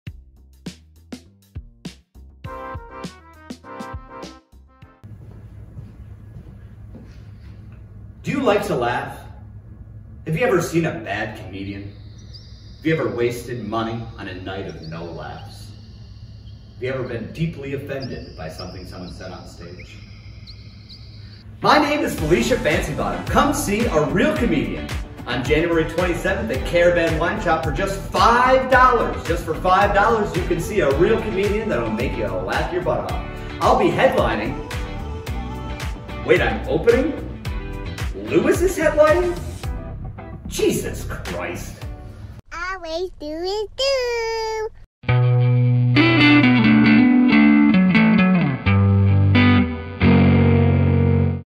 Fancybottom: Comedian Infomercial
One of the members of my comedy crew overextends his bounds before our first show.